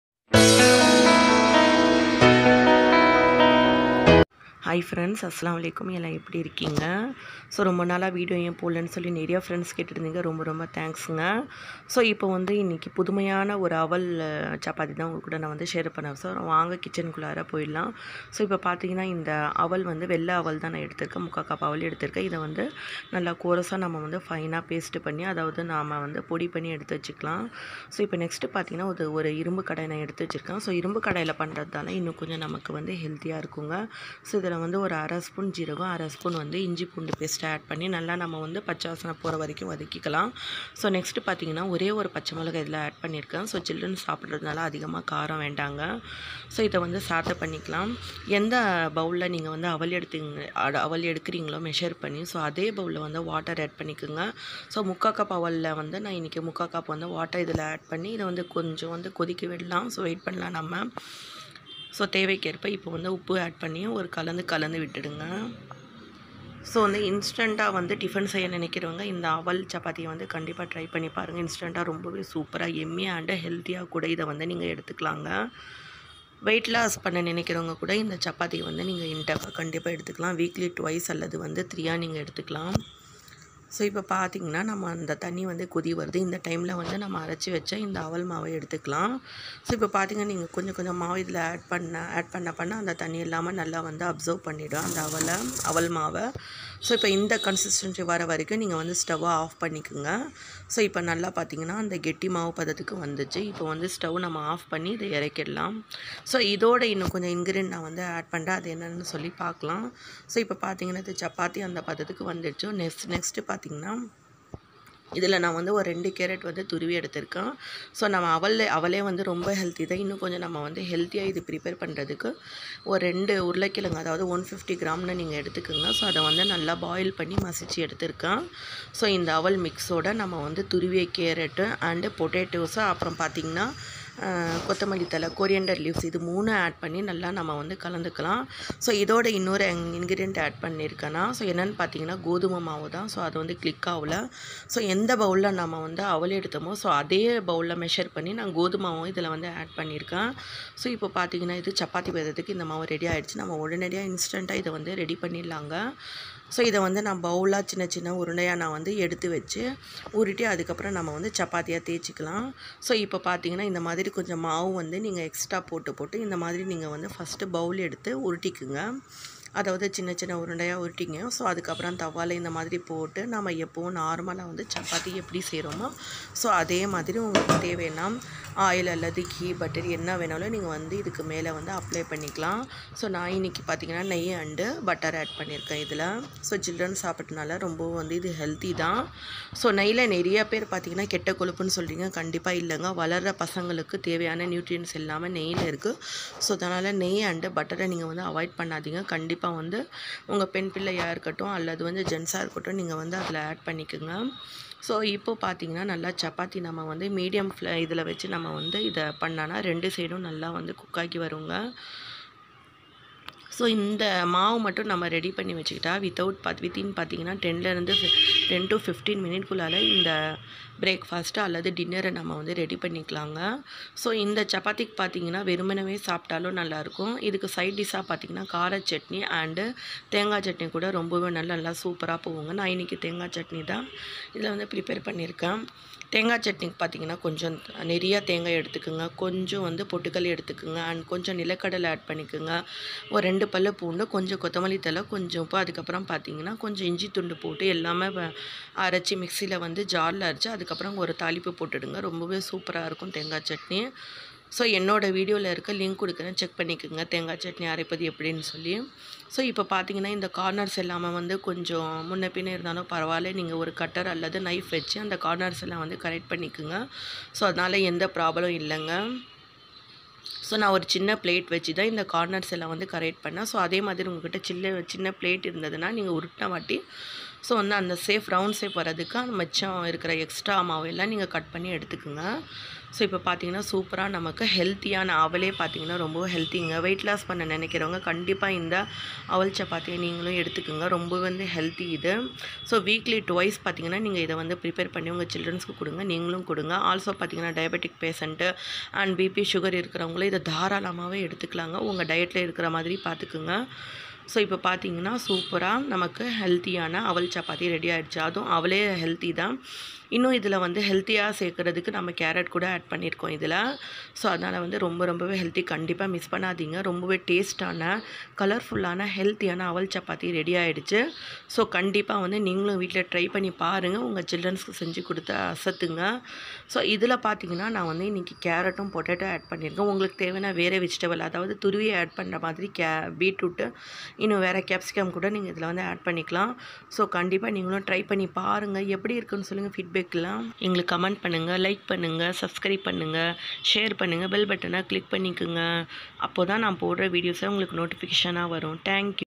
1 கப் அவல் இருந்தா 10 நிமிடத்திலே ஈஸியா செஞ்சு அசத்திடலாம்/ஹெல்த்தியான அவல் சப்பாத்தி
1 கப் அவல் இருந்தா 10 நிமிடத்திலே ஈஸியா செஞ்சு அசத்திடலாம்/ஹெல்த்தியான அவல் சப்பாத்தி